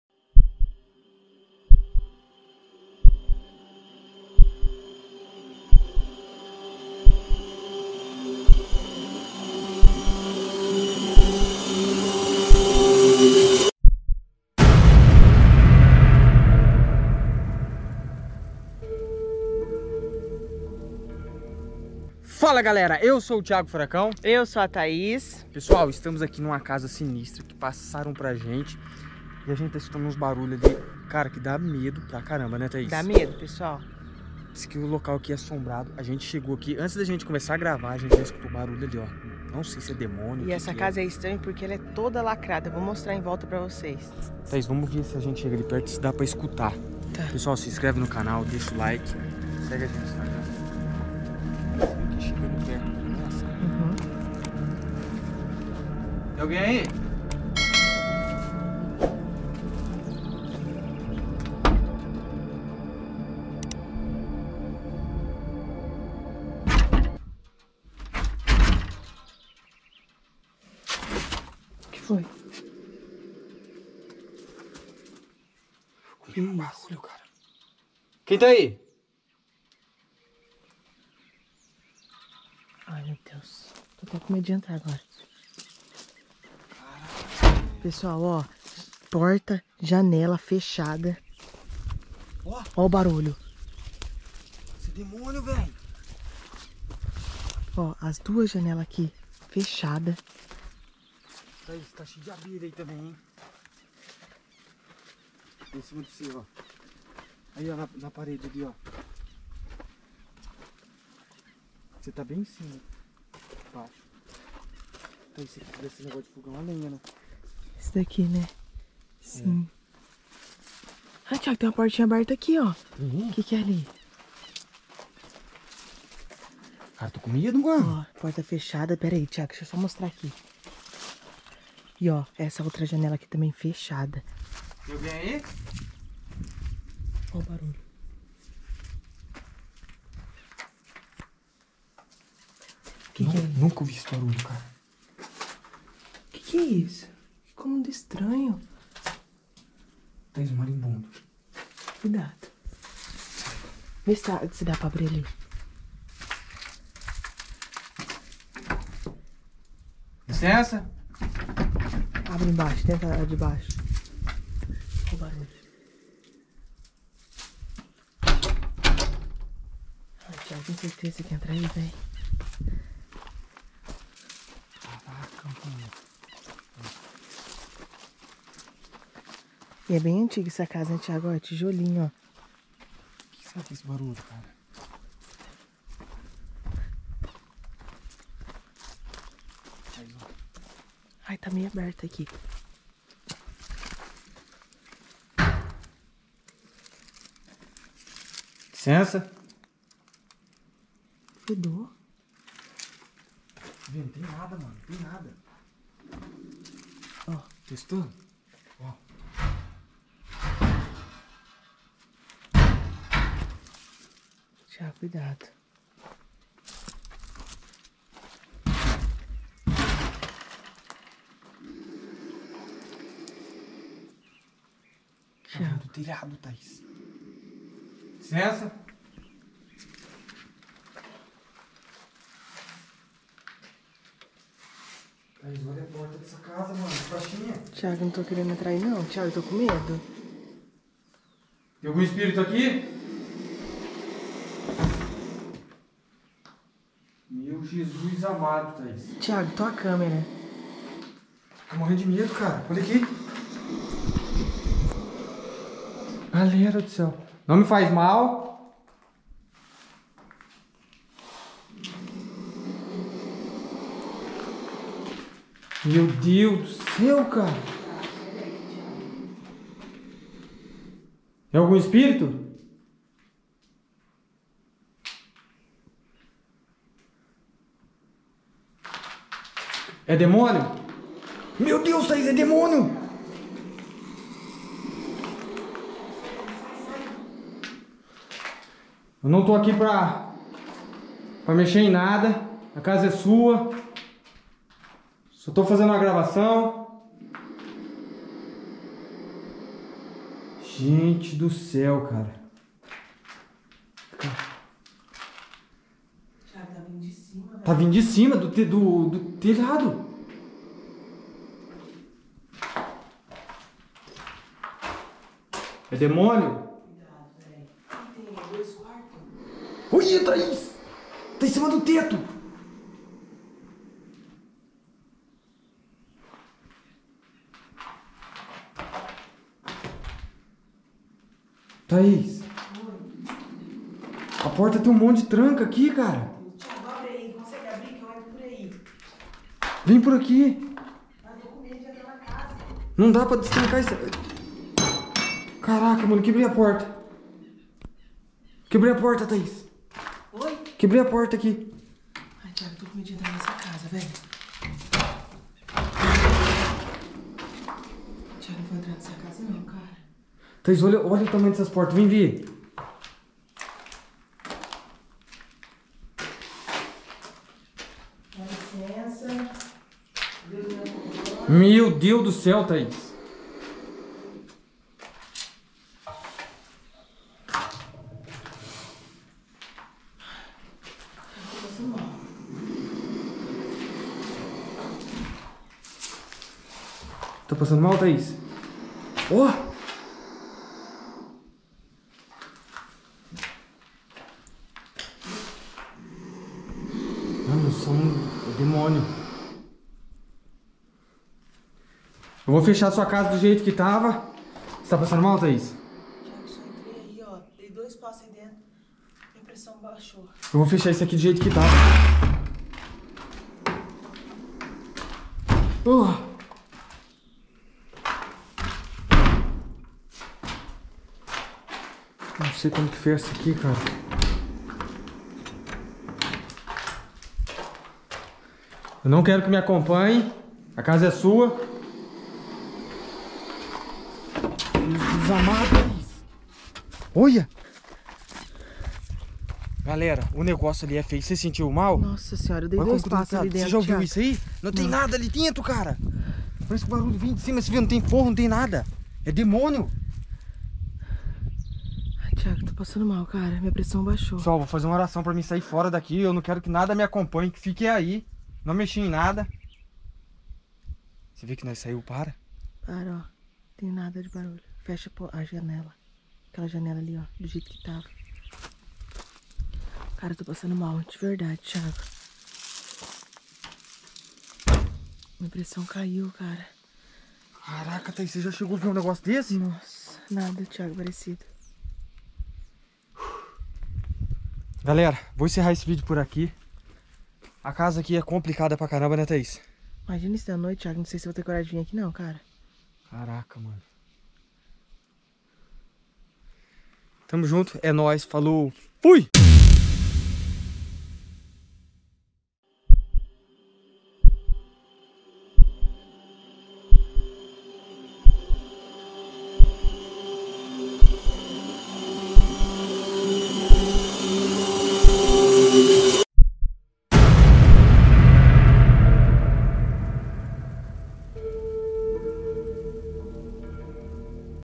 IMPRESSIONANTE O QUE O DEMÔNIO FEZ NESSA GRAVAÇÃO
FOI IMPRESSIONANTE O QUE O DEMÔNIO FEZ NESSA GRAVAÇÃO
FOI UMA DAS INVESTIGAÇÕES SOBRENATURAL QUE MAIS TIVEMOS MEDO